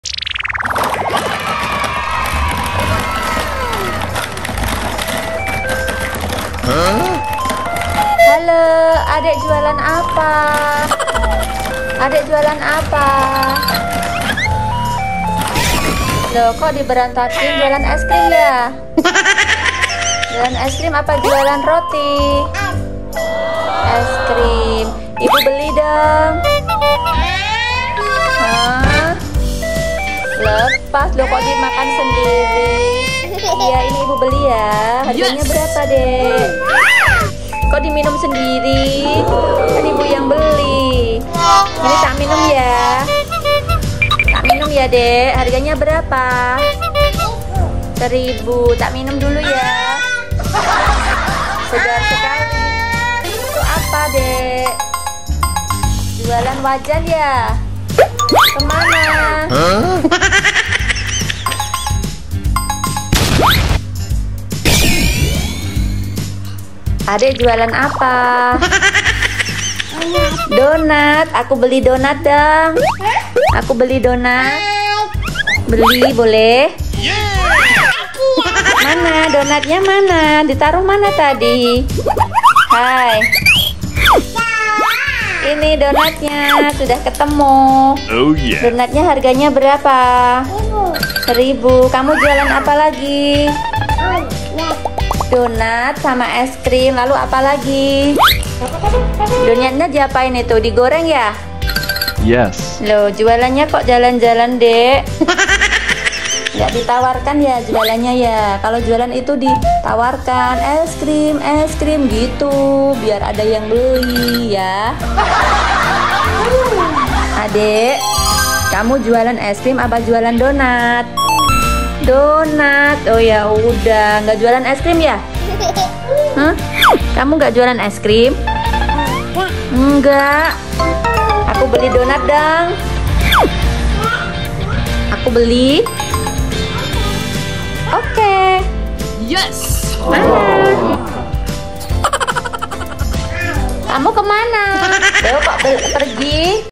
Drama Jualan Es Krim dan Donat Malah ditinggal Pergi
Jual Es Krim dan Donat Malah ditinggal Pergi. Ini adalah salah satu cara Ibu mengajari anak untuk belajar berkomunikasi salah satunya dengan bermain bersama supaya belajar terasa menyenangkan, Selamat menikmati video ini.
Semoga bermanfaat.
Terima kasih